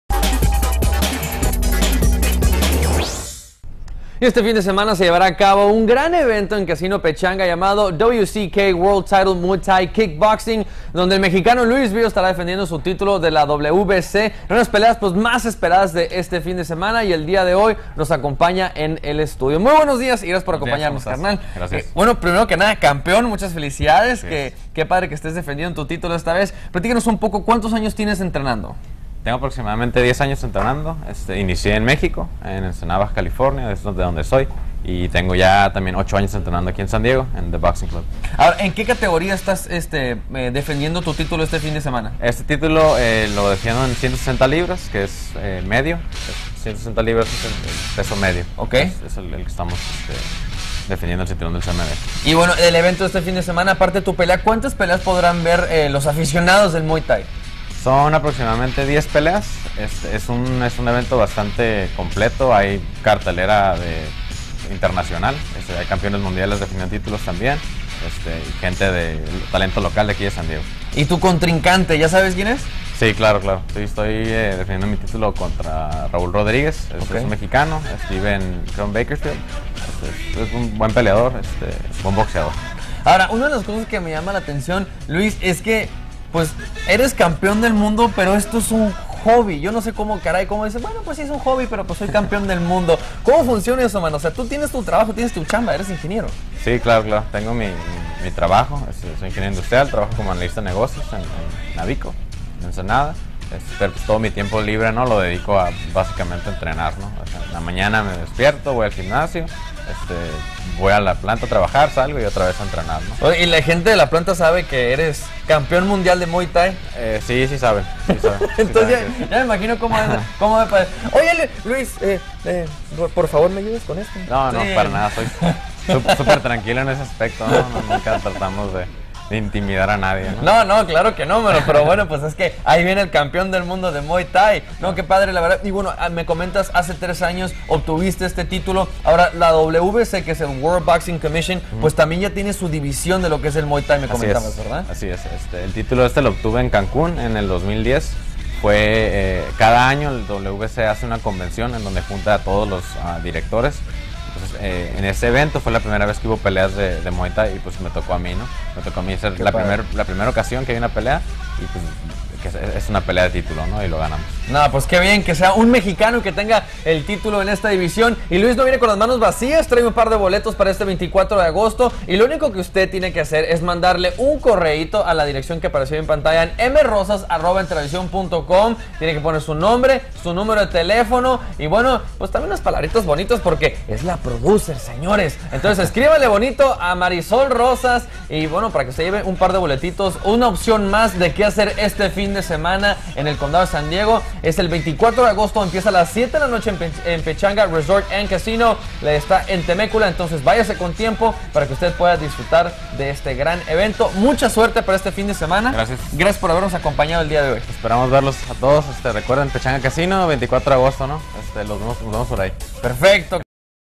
LUIS BIO, CAMPEON MUNCIAL DE MUAY THAI
(Entravision)SAN DIEGO.-Este fin de semana se llevará a cabo un gran evento en el casino Pachanga llamado WCK World Title Muay Thai Kickboxing donde el mexicano Luis Bio estará defendiendo su titulo de la WBC en una de las peleas más esperadas de este fin de semana y el día de hoy nos acompaña el campeón en el estudio.